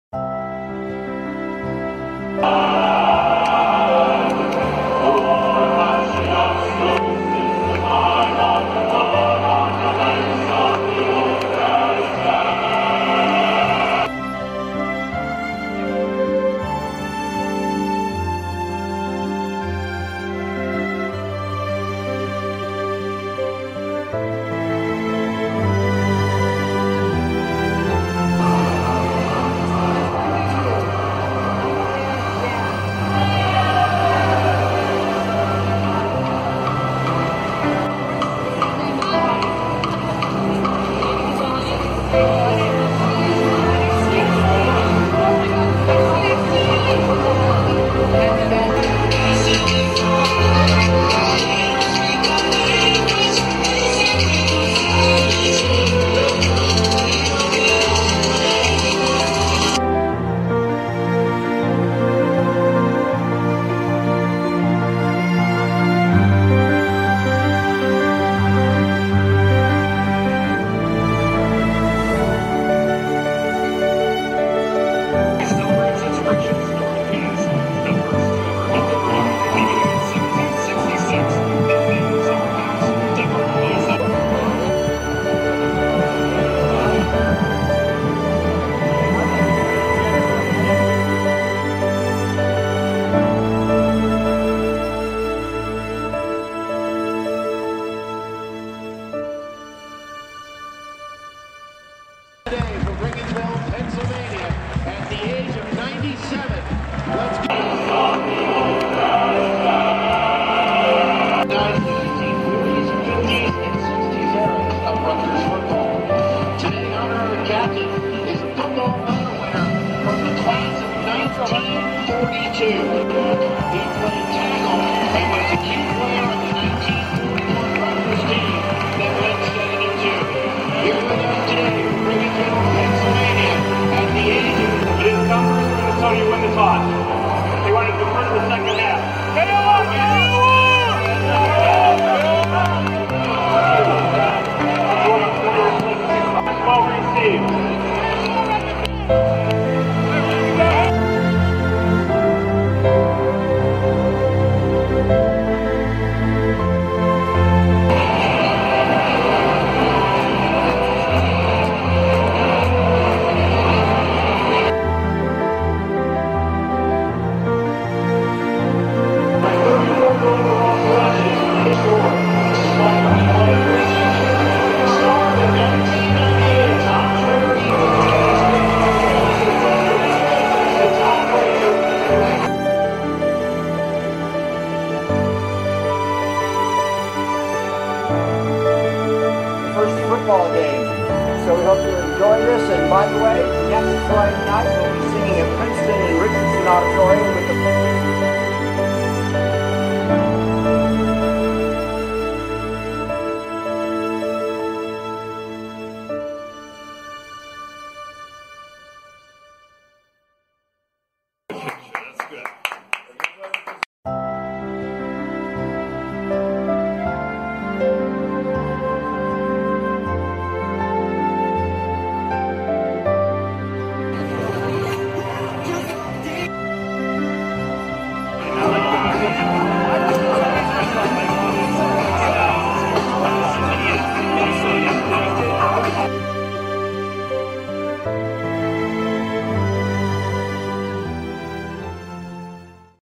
Rutgers University homecoming. 150th anniversary of college football.
Gilbert Greenberg.  At 97 the oldest Rutgers football letterman.